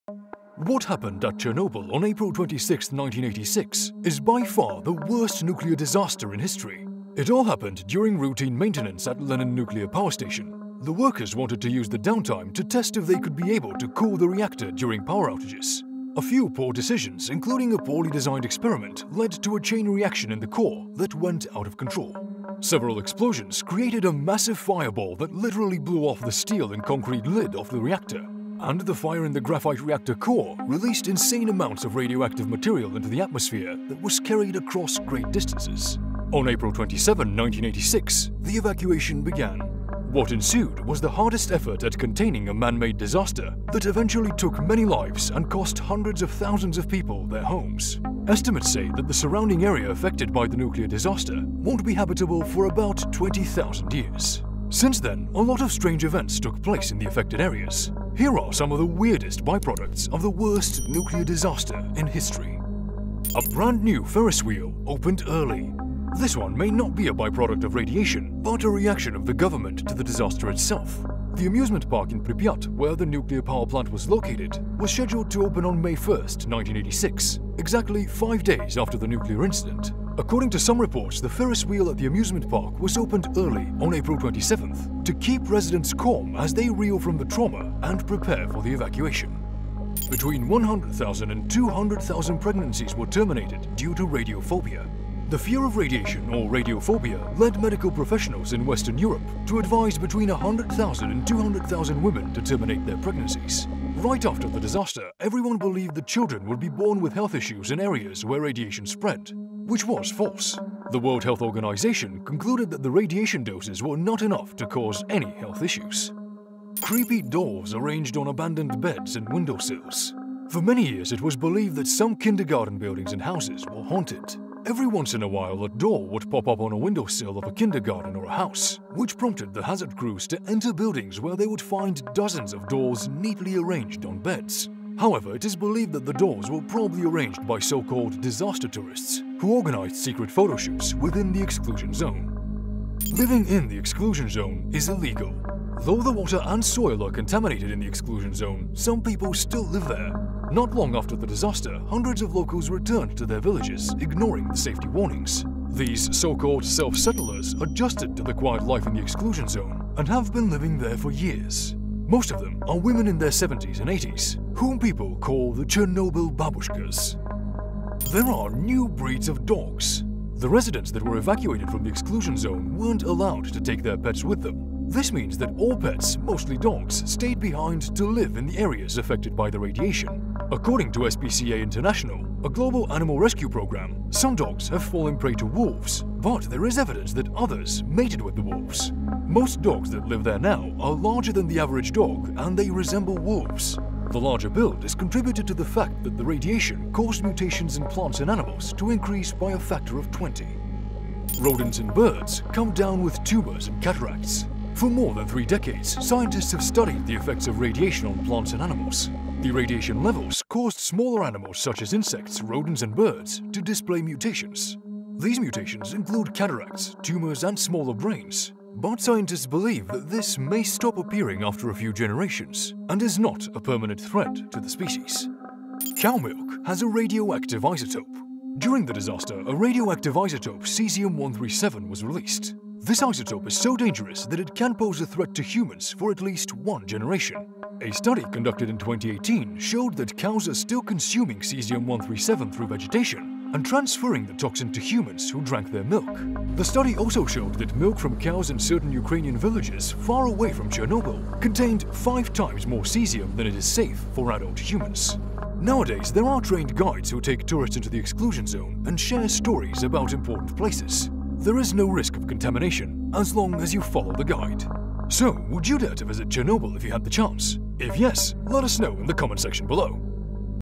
Weird Things Caused By Chernobyl Nuclear Disaster
What happened at Chernobyl on April 26, 1986, is by far the worst nuclear disaster in history. It all happened during routine maintenance at Lenin Nuclear Power Station; the workers wanted to use the downtime to test if they would be able to cool the reactor during power outages. A few poor decisions, including a poorly designed experiment, lead to a chain reaction in the core that went out of control.

Several explosions created a massive fireball that literally blew off the steel and concrete lid of the reactor, and the fire in the graphite reactor core released insane amounts of radioactive material into the atmosphere that was carried across great distances. On April 27, 1986, the evacuation began.

What ensued was the hardest effort at containing a man-made disaster that eventually took many lives and cost hundreds of thousands of people their homes. Estimates say that the surrounding areas affected by the nuclear disaster won’t be habitable for about 20,000 years.

Since then, a lot of strange events took place in the affected areas. Here are some of the weirdest byproducts of the worst nuclear disaster in history.